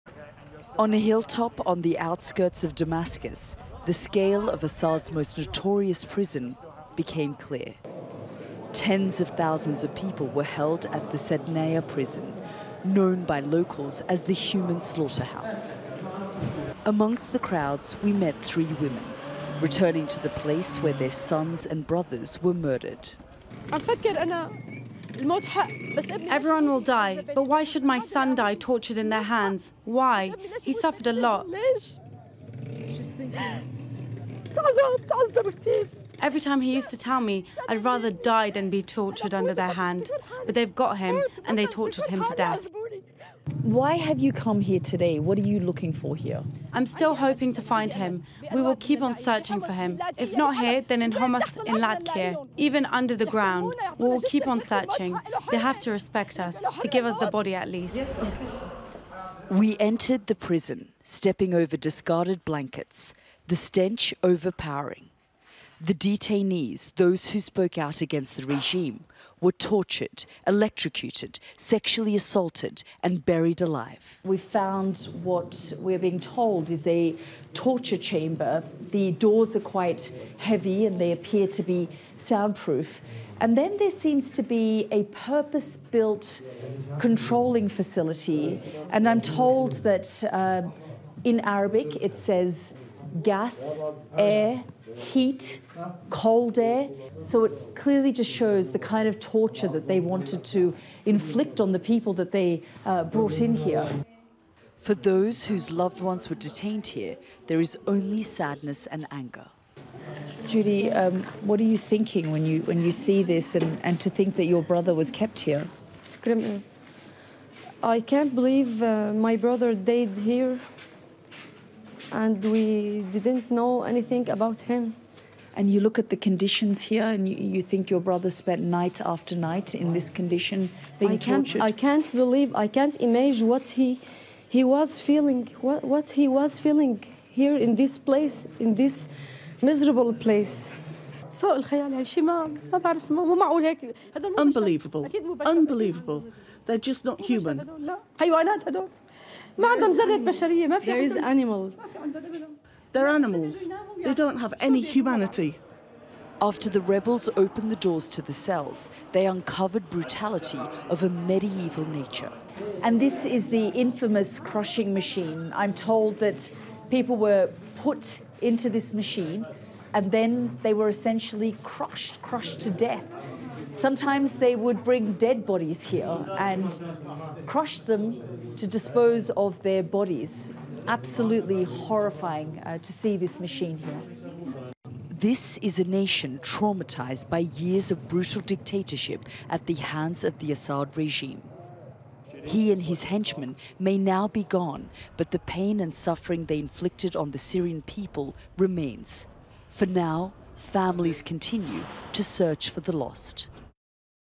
The 'iron press': How the Assad regime tortured its victims to death - then crushed their bodies
Sky's Yalda Hakim goes into the notorious Sednaya prison on the outskirts of Damascus.

For years, we have heard claims of mass executions taking place there, and torture being used on inmates.

As we walked through the doors of the prison, we got a window into that horrific back story.